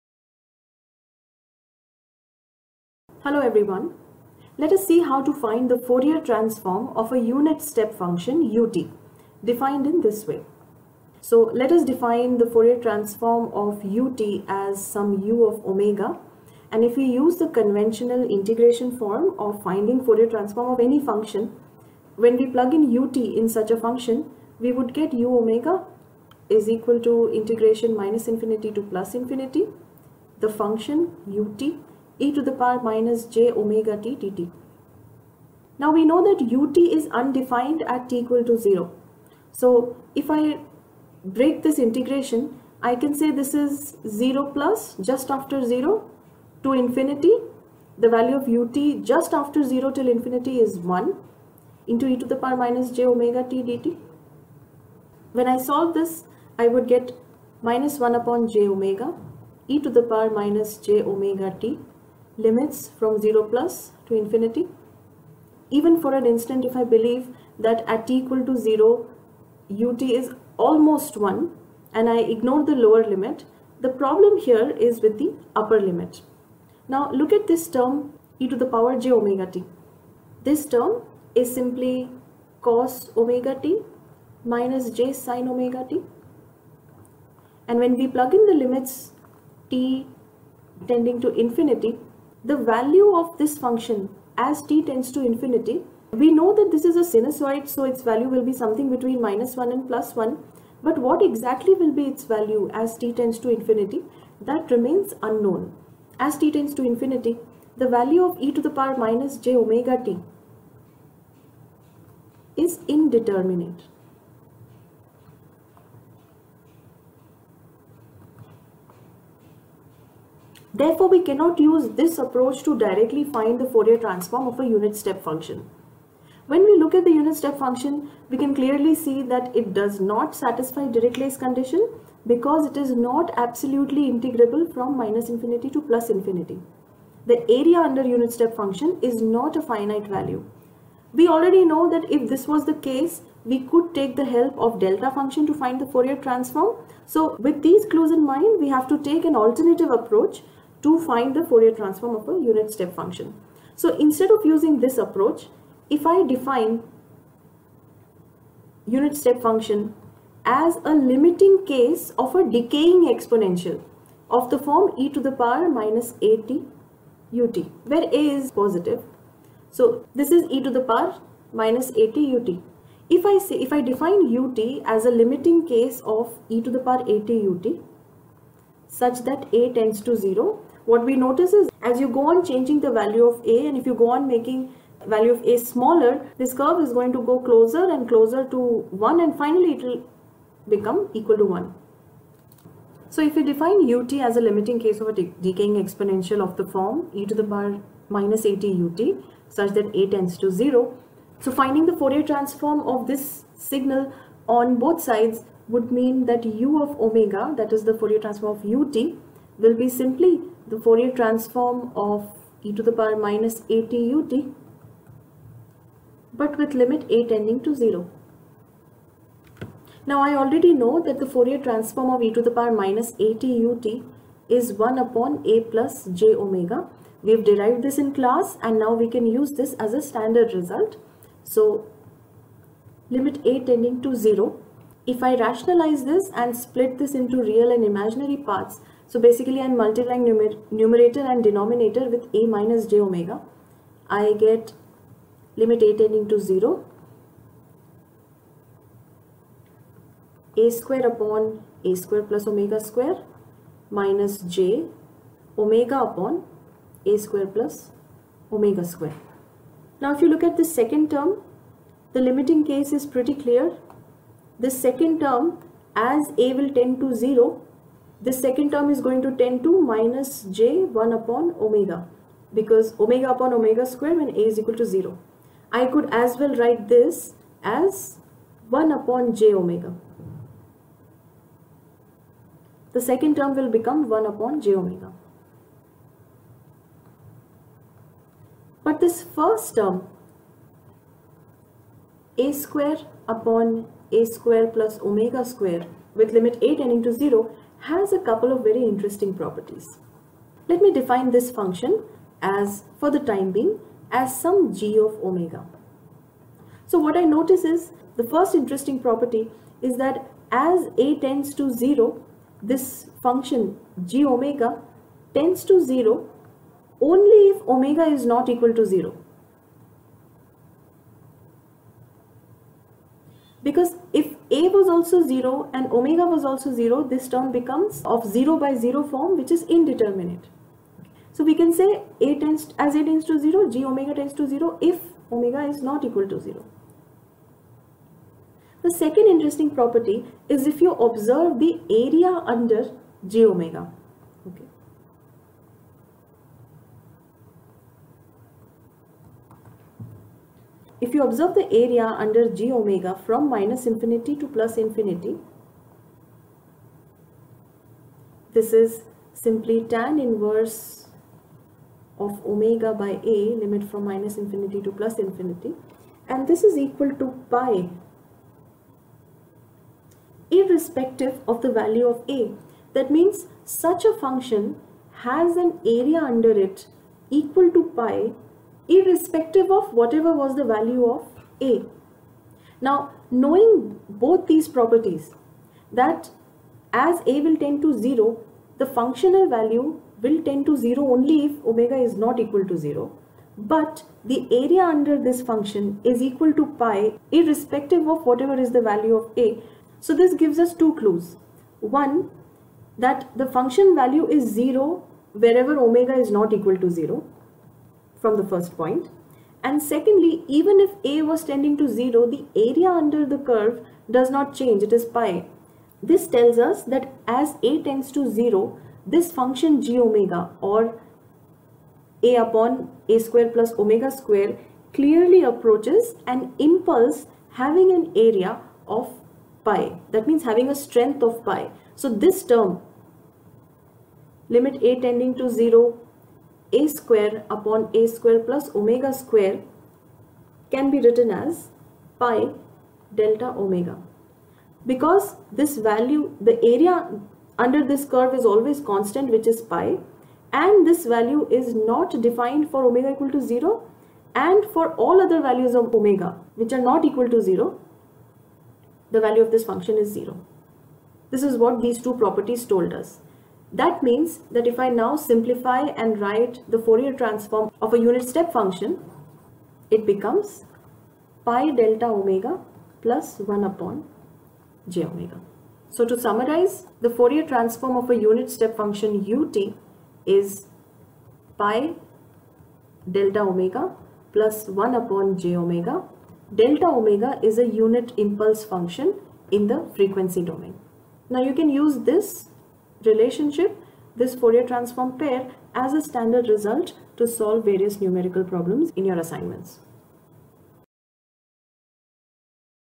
Lecture 06 Fourier Transform of Unit Step Function - Dr. Rajlaxmi Chouhan, IIT Jodhpur
The Fourier transform of a unit step function u(t) is derived using an approximation approach.
Erratum:
4:10 It should be a / (a^2 + omega^2) (Incorrectly written as a^2 in the numerator)
5:00 Again, g(omega) is a / (a^2 + omega^2) (Incorrectly witten as a^2/ (a^2 + omega^2)
Reference: BP Lathi, Signal Processing and Linear Systems, Oxford University Press, 1998